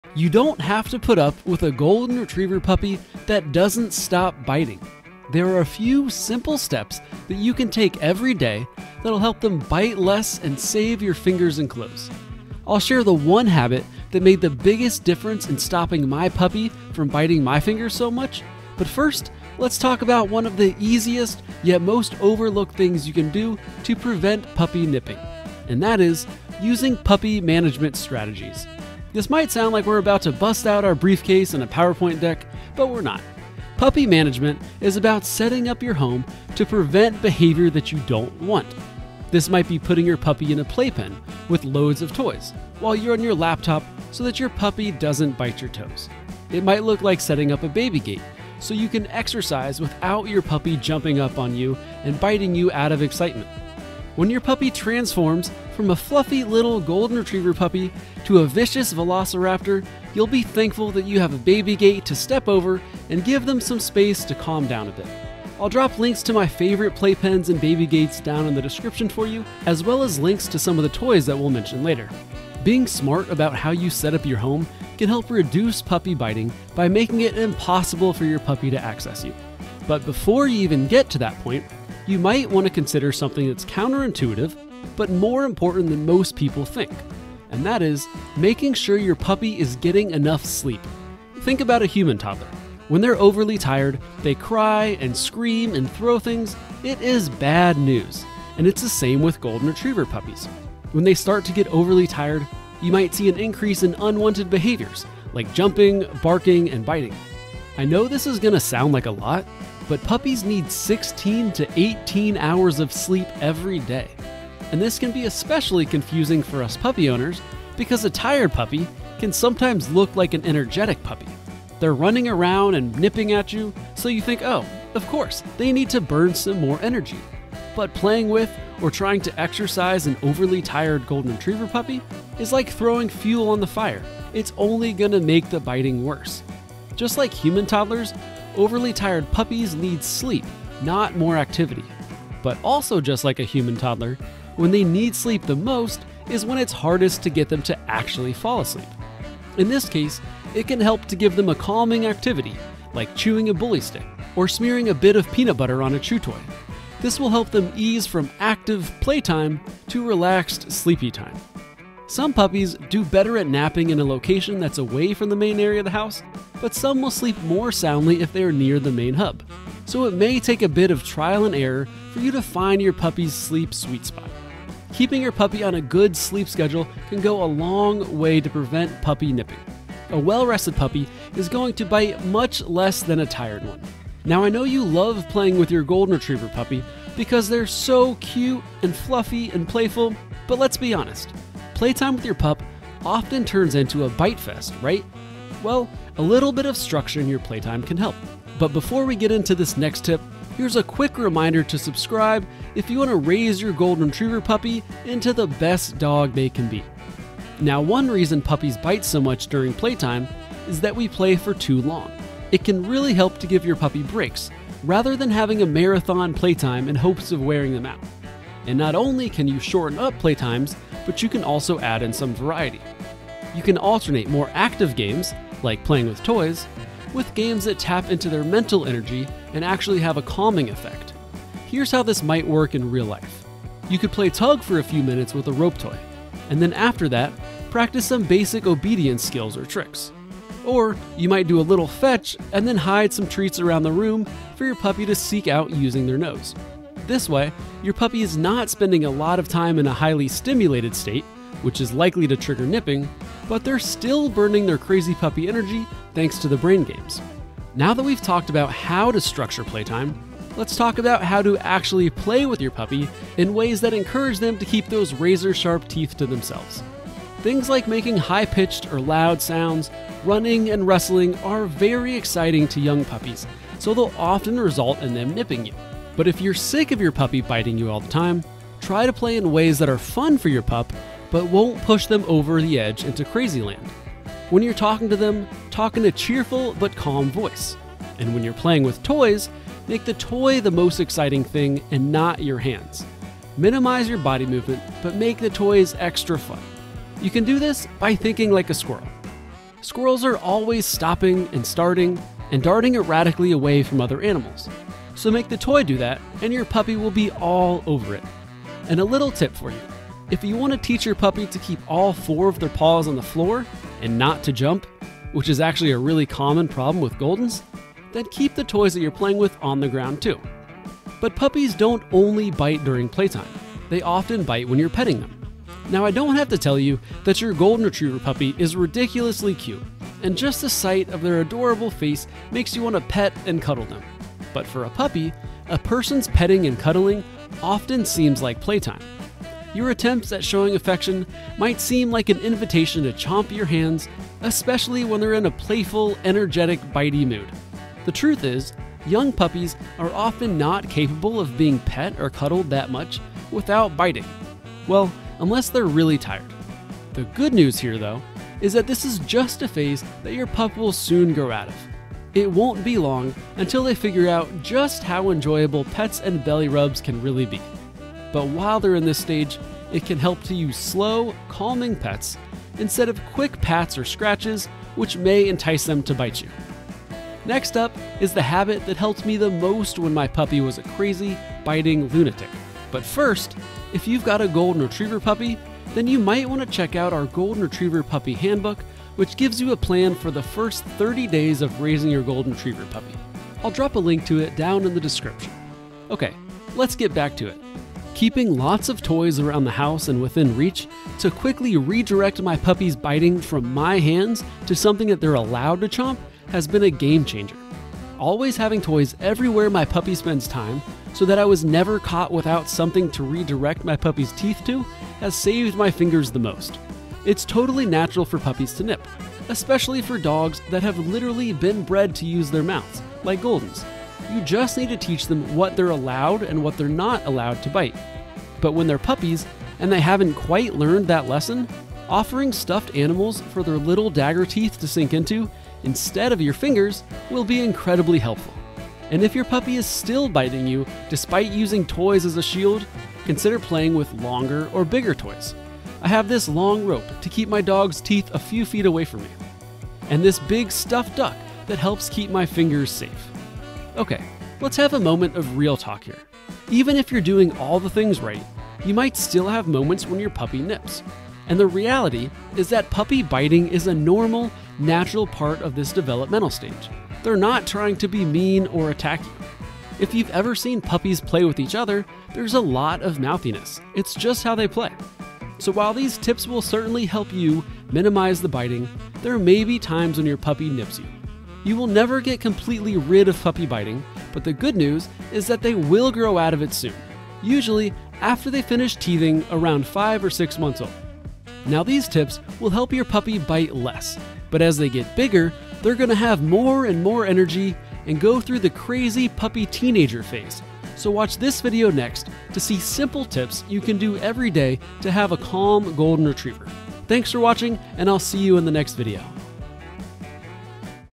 Do This Every Day To Stop Golden Retriever Puppy Biting
Golden Retriever puppies naturally bite a lot, but you don’t need to suffer through it helplessly.

There are a few simple things you can do every day to help your Golden Retriever puppy not bite your fingers and clothes so much.

See what those things are in this video.

Mentioned links:

Baby gate

Play pen

Rope toy

Stuffed duck